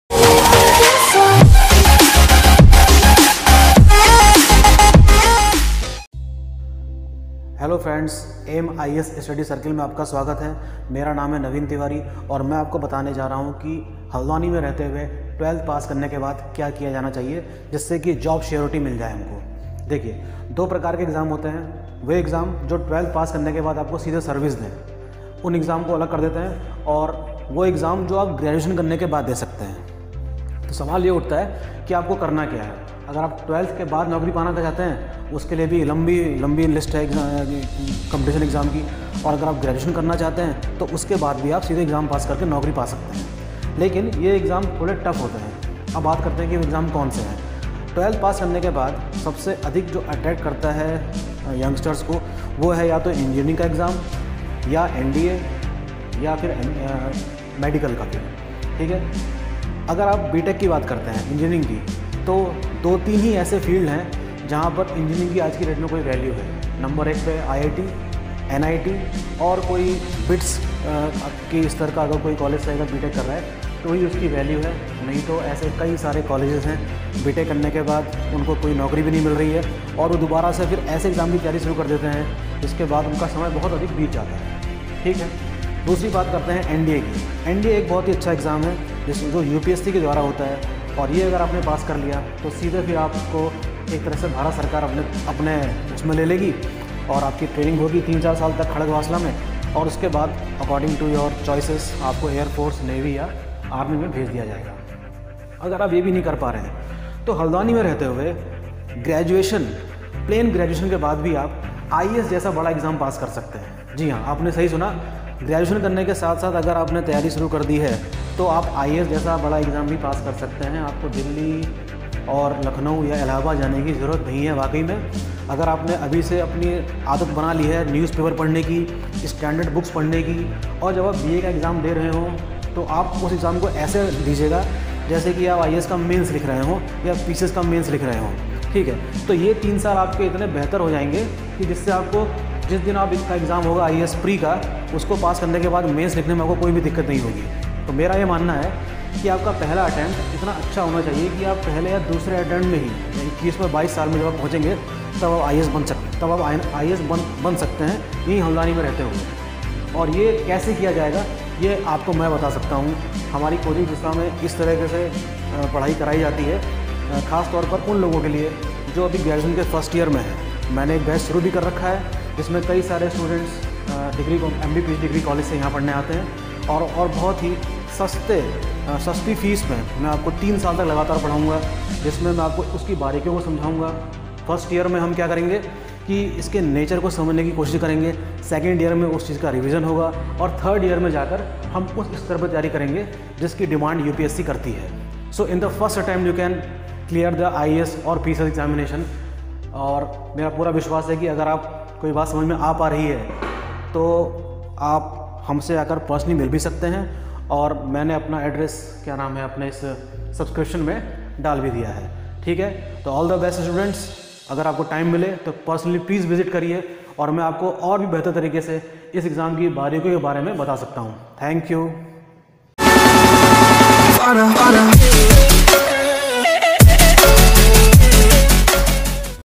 Must watch video for Haldwani students .
This video is mainly for those who want to shape their career in Civil Services (IAS/PCS).
. The only English medium coaching for the civil services in haldwani.
. Address :- 2nd Floor Bigmo's Restaurant Behind Degree College , Haldwani - Uttrakhand